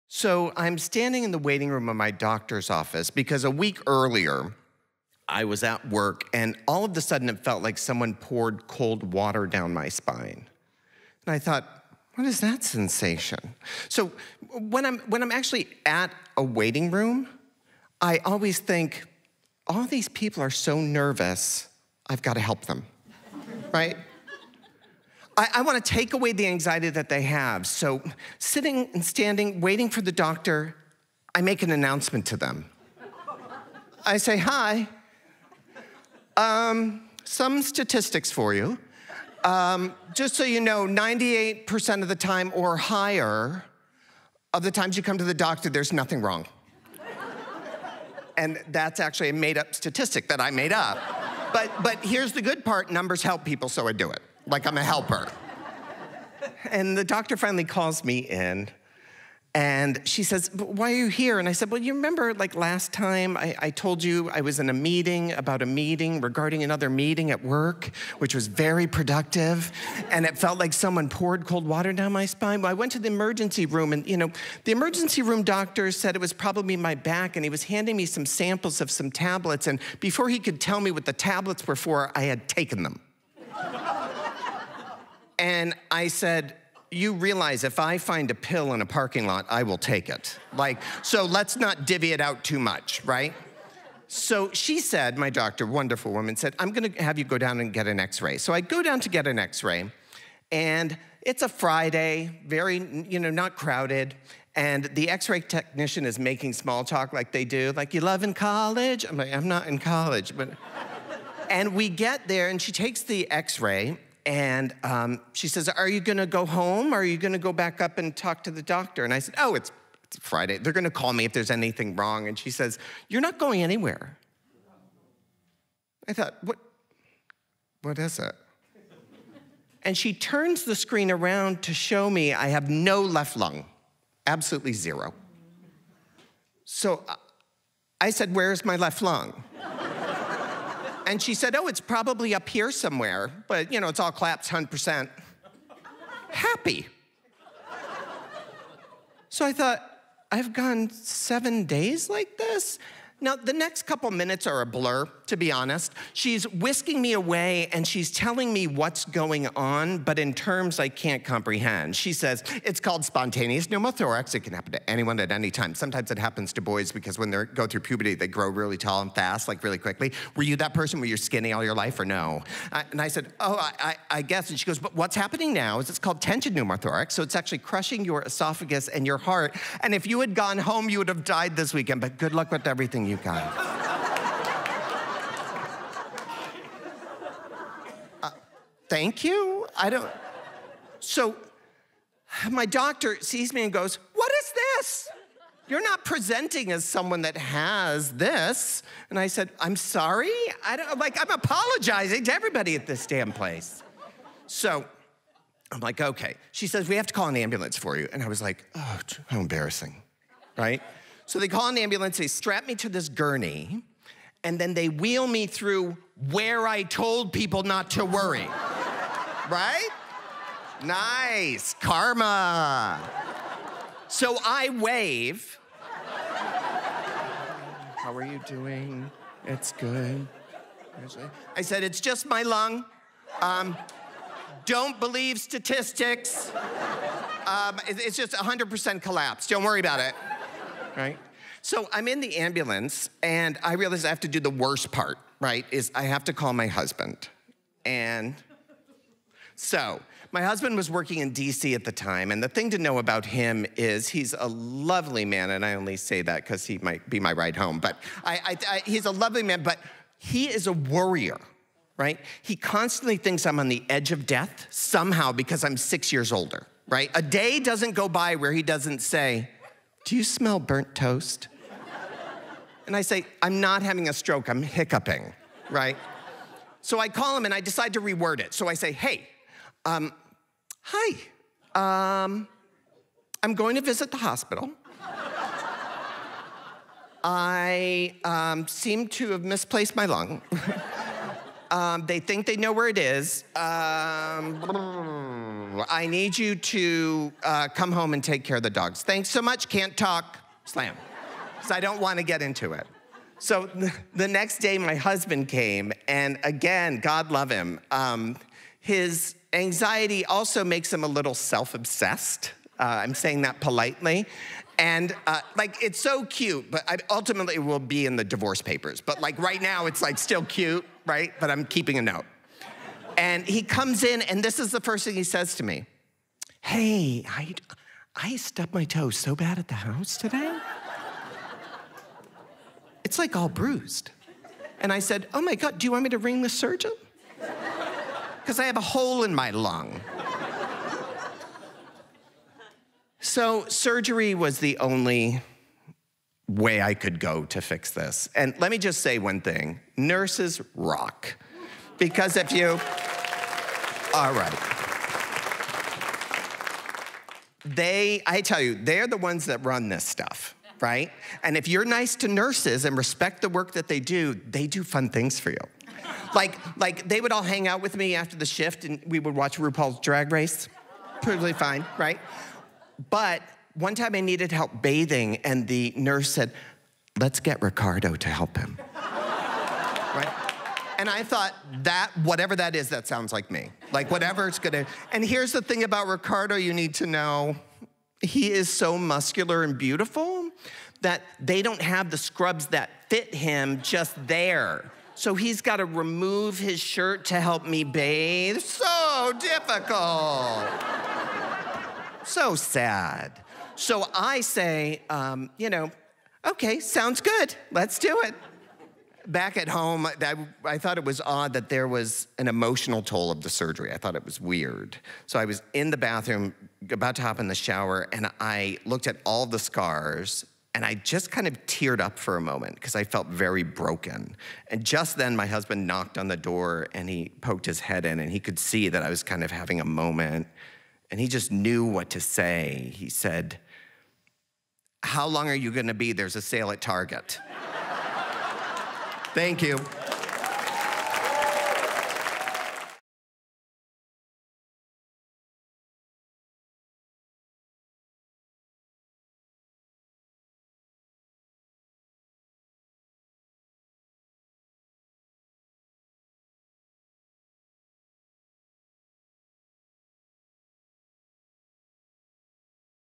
Tone Nunes's Collapsed Lung Beats The "Statistics"
Tone Nunes shares the funny side of his hospital stay.